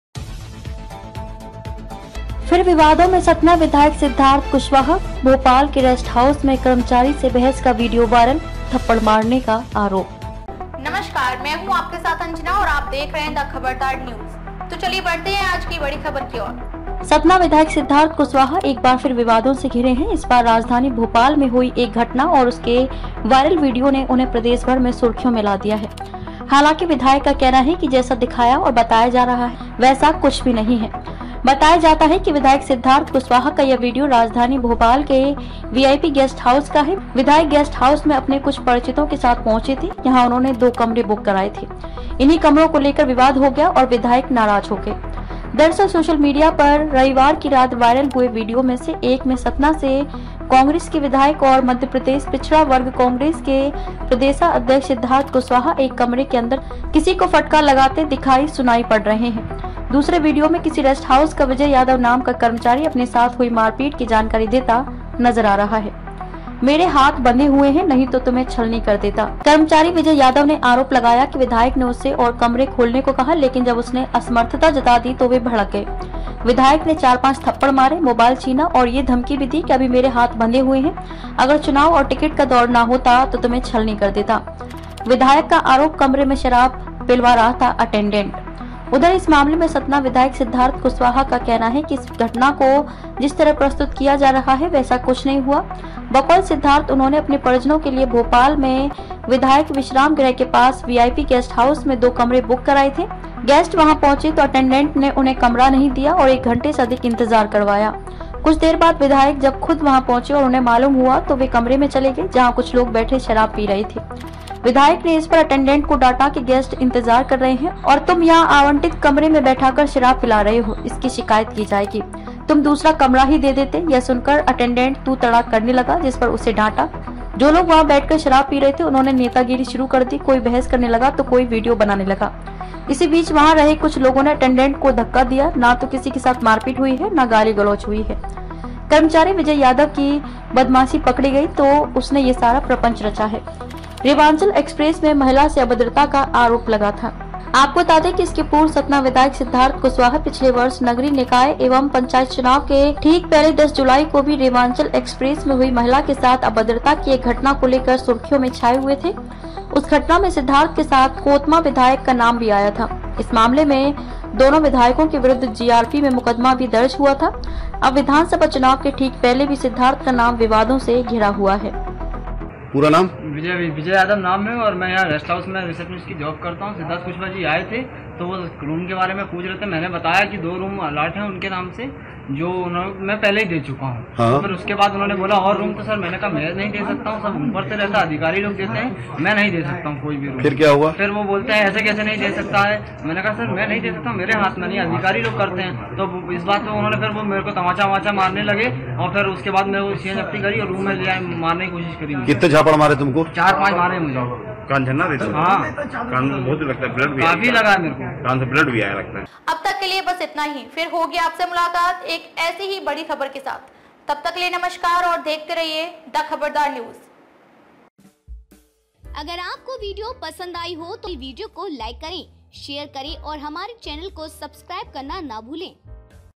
Bhopal: VIP गेस्ट हाउस कांड मे घिरे, विधायक Siddharth Kushwaha। Satna News। MP News.
Satna News:-सतना विधायक सिद्धार्थ कुशवाहा एक बार फिर विवादों से घिरे हैं। इस बार राजधानी भोपाल में हुई एक घटना और उसके वायरल वीडियो ने उन्हें प्रदेश भर में सुर्खियों में ला दिया है। हालांकि विधायक का कहना है कि जैसा दिखाया और बताया जा रहा है। वैसा कुछ भी नहीं है। बताया जाता है कि विधायक सिद्धार्थ कुशवाहा का यह वीडियो राजधानी भोपाल के वीआईपी गेस्ट हाउस का है। विधायक गेस्ट हाउस में अपने कुछ परिचितों के साथ पहुंचे थे। यहां उन्होंने दो कमरे बुक कराए थे। इन्हीं कमरों को लेकर विवाद हो गया और विधायक नाराज हो गए।
दरअसल, सोशल मीडिया पर रविवार की रात वायरल हुए वीडियो में से एक में सतना से कांग्रेस के विधायक और मप्र पिछड़ा वर्ग कांग्रेस के प्रदेशाध्यक्ष सिद्धार्थ कुशवाहा एक कमरे के अंदर किसी को फटकार लगाते दिखाई- सुनाई पड़ रहे हैं। दूसरे वीडियो में किसी रेस्ट हाउस का विजय यादव नाम का कर्मचारी अपने साथ हुई मारपीट की जानकारी देता नजर आ रहा है।